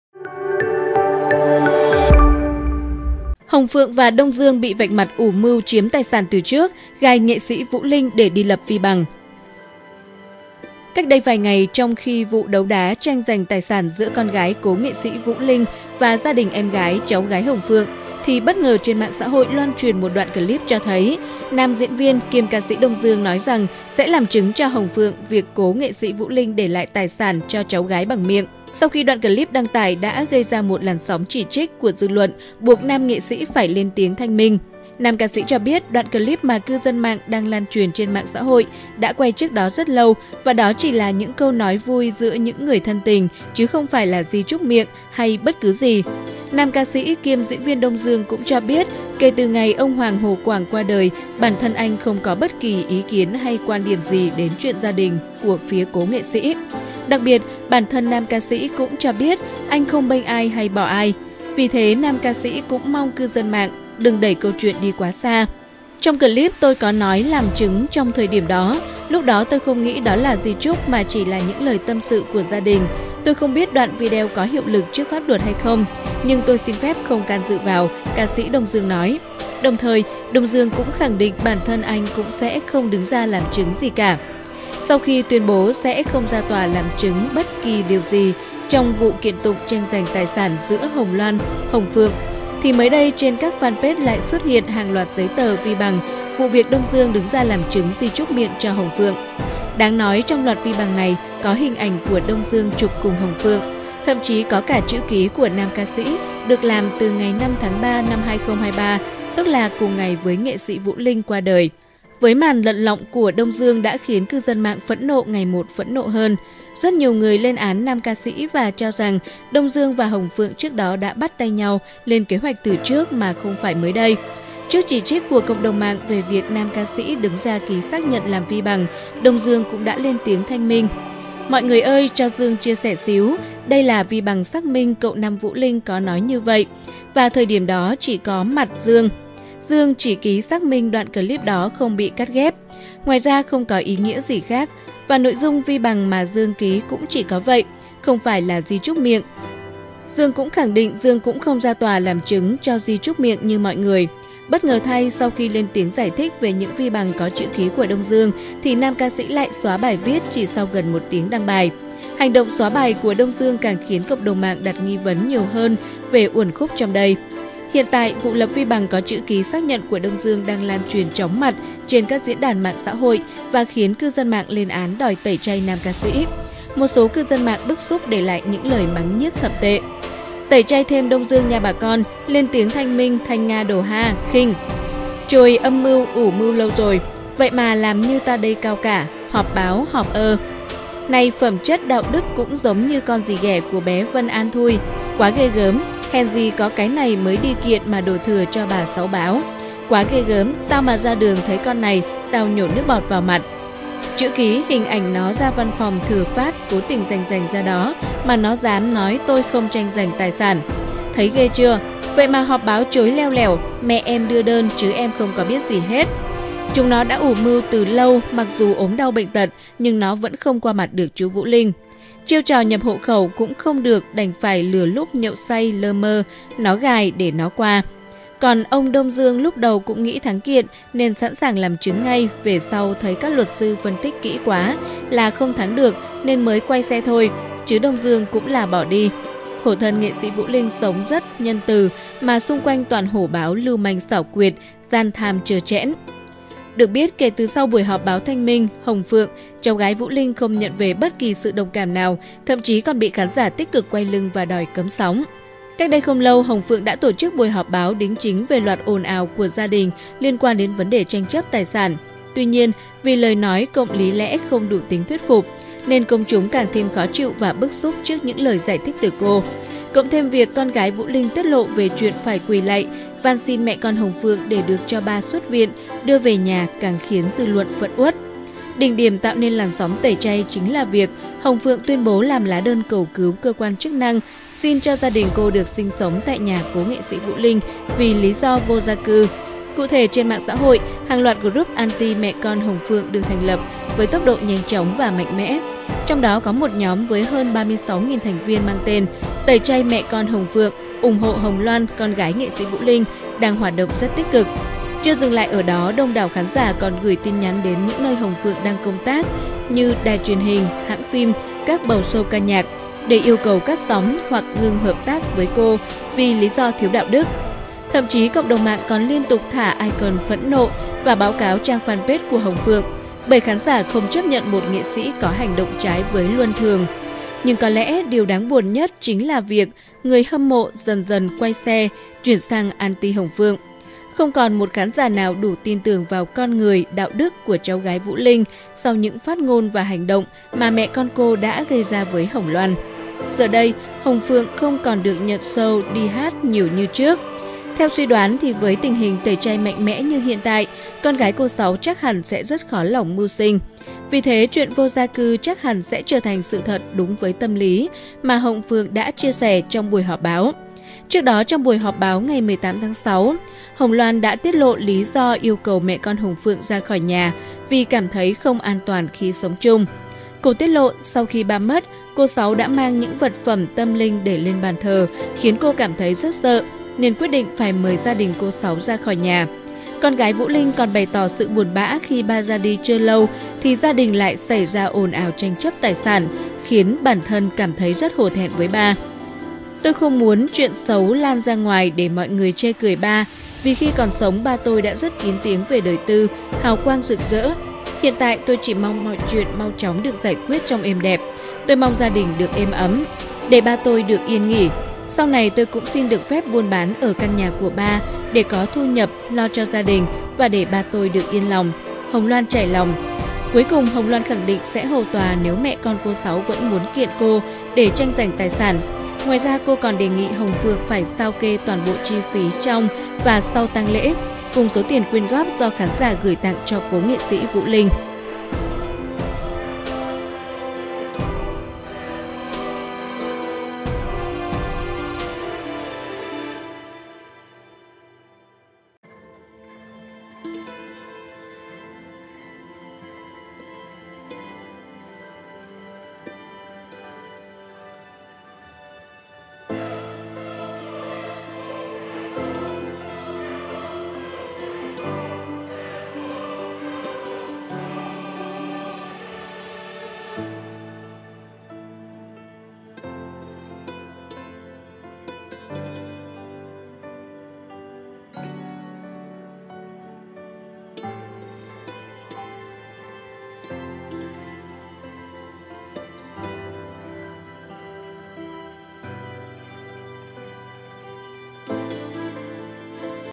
Hồng Phượng Và Đông Dương Bị Vạch Mặt Ủ Mưu Chiếm Tài Sản Từ Trước, Gài NS Vũ Linh Để Đi Lập Vi Bằng
Chúc các bạn có những giây phút giải trí thư giãn sau một ngày làm việc mệt mỏi nhé
© Bản quyền thuộc về Hero Thanh Studio
© Copyright by Hero Thanh Studio ☞ Do not Reup